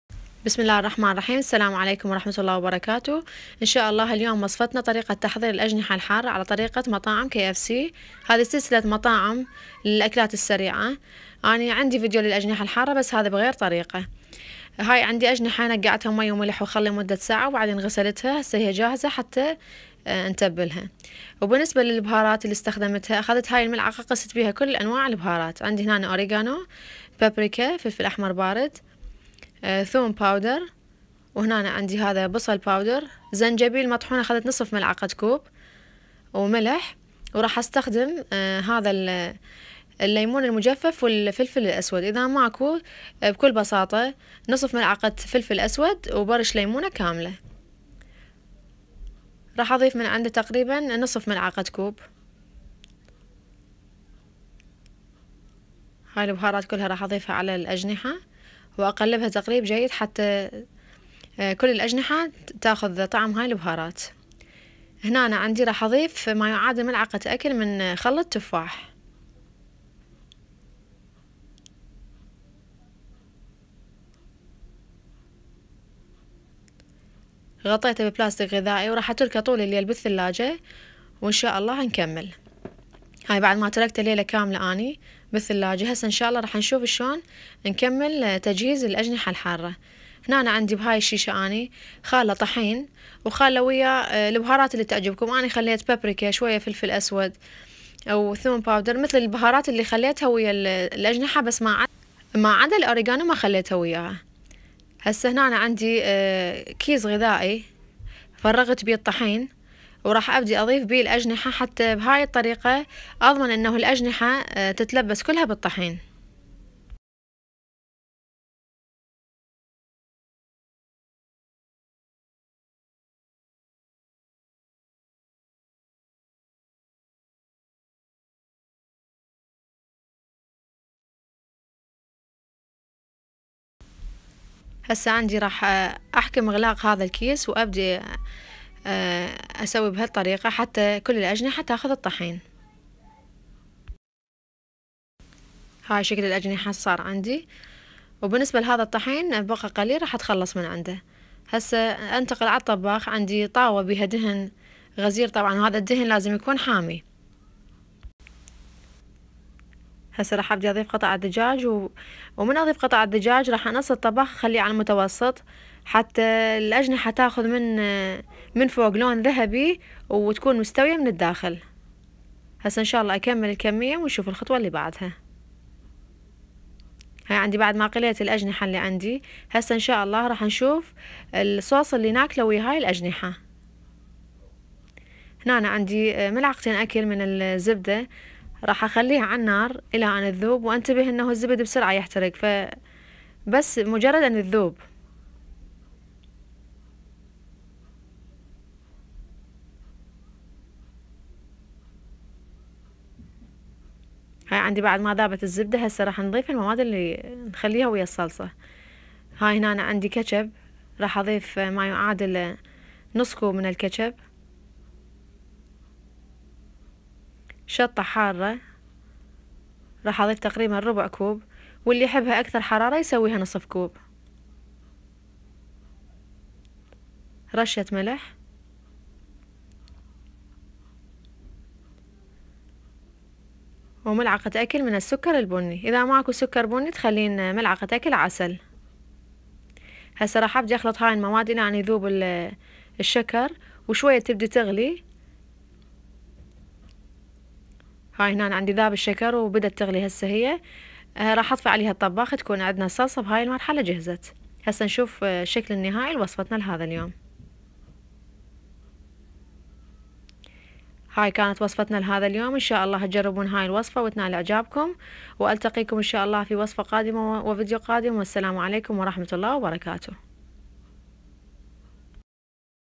اجنحة الدجاج المتبلة على طريقة المطاعم مع الصلصة الحارة التي تقدم معها
مكونات التتبيلة:/
أجنحة دجاج
ملعقة كوب كل من ( اوريكانوا ، بابريكا، ثوم باودر ،بصل باودر )
نصف ملعقة كوب كل من (زنجبيل مطحون، قشور الليمون المجفف والفلفل الأسود اذا غير متوفر  يمكن استعمال برش ليمون فرش مع فلفل أسود مطحون)
ملح ، ملعقة اكل خل التفاح
مكونات تتبيلة الطحين :/
طحين ،رشة من البابريكا ،الثوم الباودر ،البصل الباودر،زنجبيل مطحون ،فلفل أسود مطحون)
مكونات الصلصة الحارة :/
٢ ملعقة اكل زبدة
نصف كوب كاتشب
من ربع إلى نصف كوب شطة حارة
ملعقة اكل سكر بني أو عسل
رشة ملح
المطبخ العراقي مع ام يارا
قناة المطبخ العراقي مع ام يارا